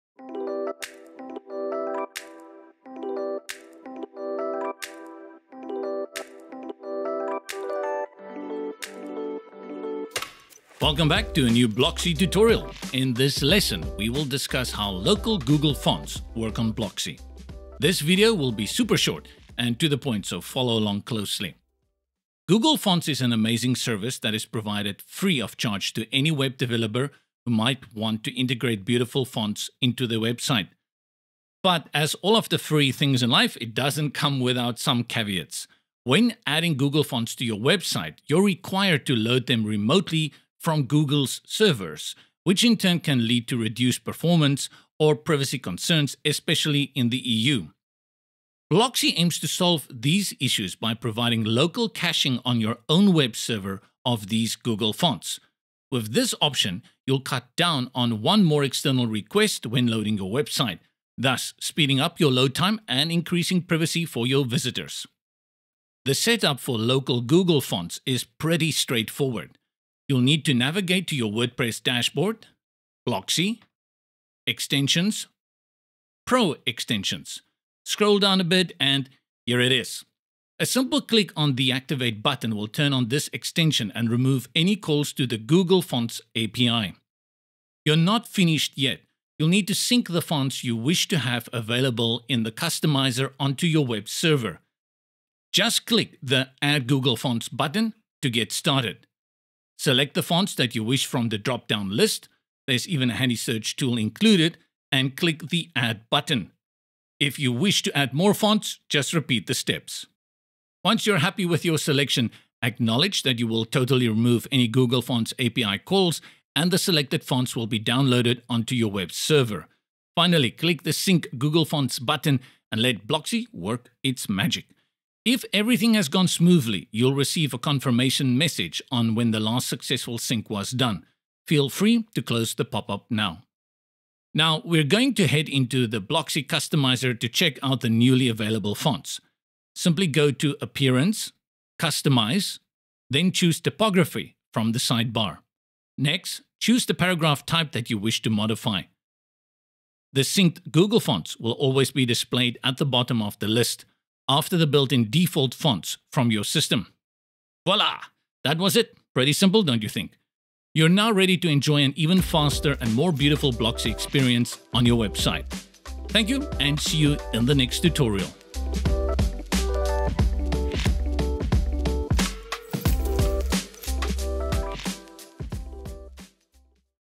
Local Google Fonts | Blocksy Premium
Google Fonts is an amazing service that is provided free of charge to any web developer who might want to integrate beautiful fonts into their website. But, as all the free things in life, it doesn’t come without some caveats. When adding Google Fonts to your website, you’re required to load them remotely from Google’s servers, which in turn can lead to reduced performance, or privacy concerns, especially in the EU.

In this lesson, we will discuss how Local Google Fonts work on Blocksy. This video will be super short and to the point, so follow along closely.